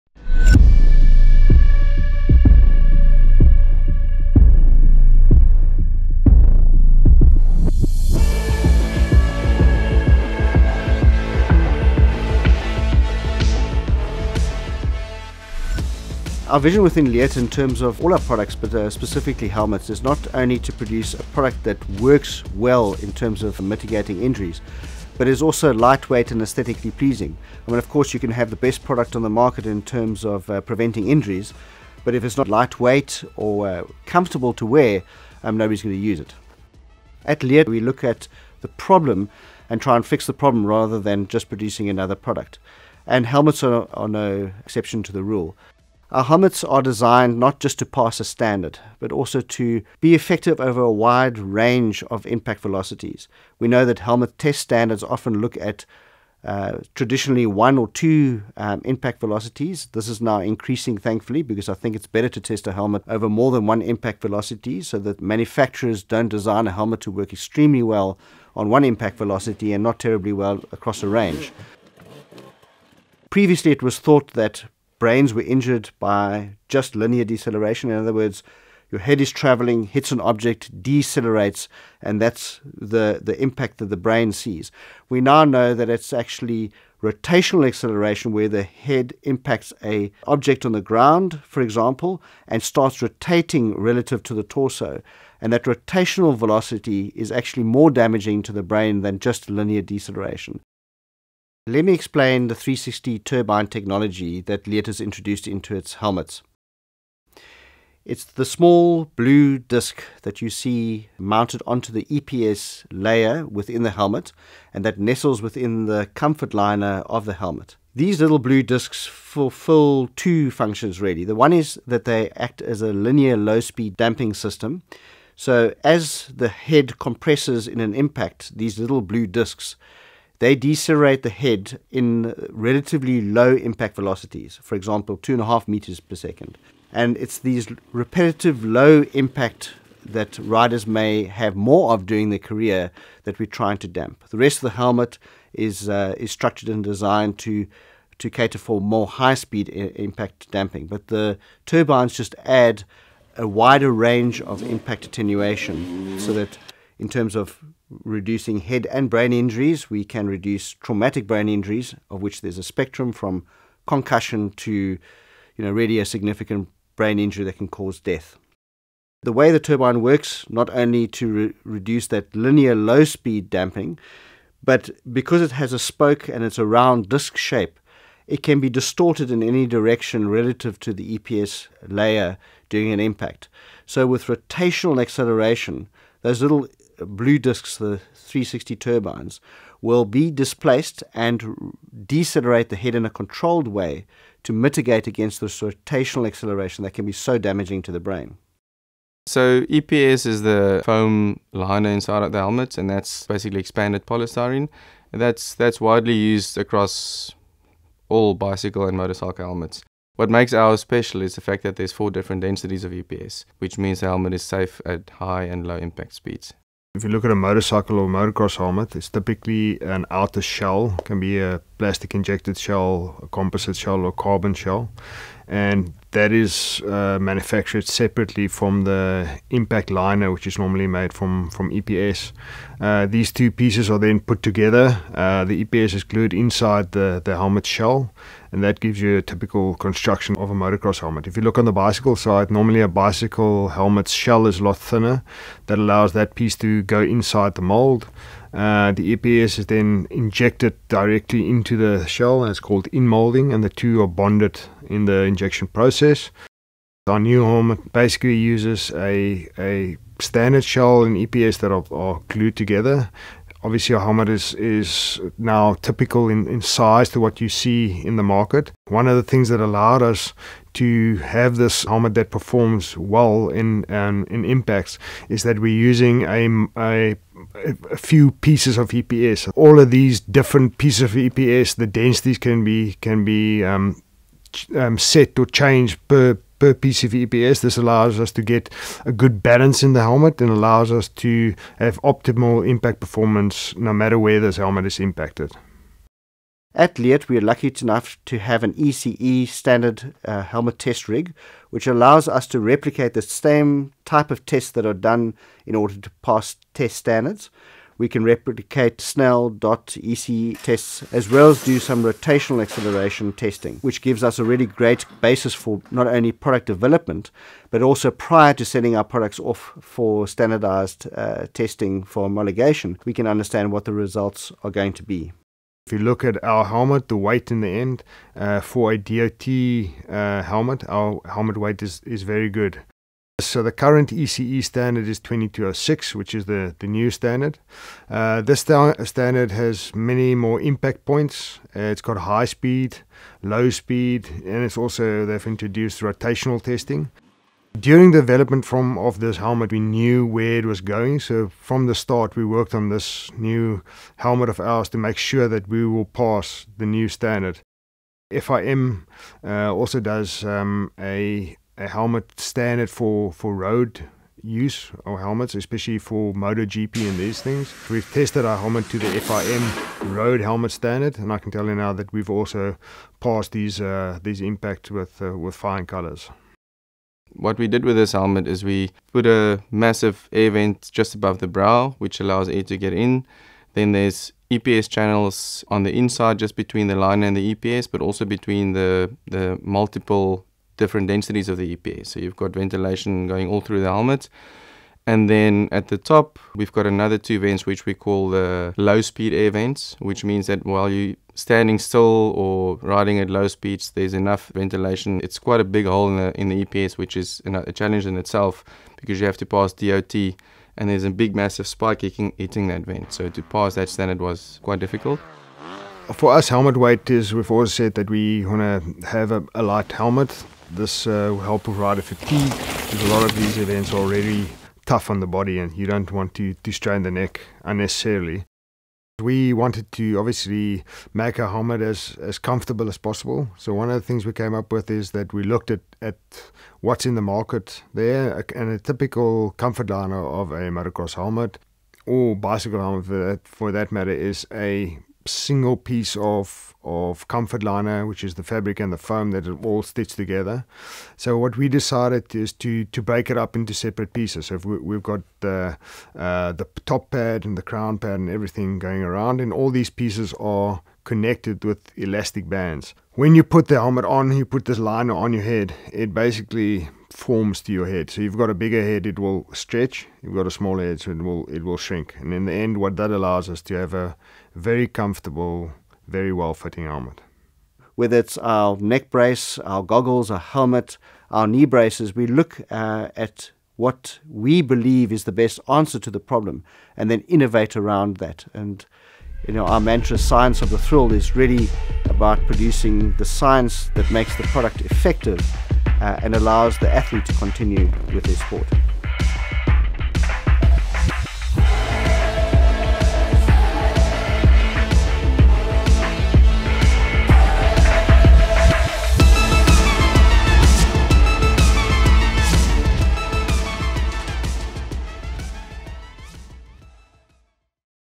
Leatt Heritage - S1 E4 - Helmets
“Our vision within Leatt, in terms of all our products, but specifically helmets is not only to produce a product that works well in terms of mitigating injuries, but is also lightweight and aesthetically pleasing. Of course, you can have the best product on the market in terms of preventing injuries, but if it’s not lightweight or comfortable to wear, nobody’s going to use it” – Dr Chris Leatt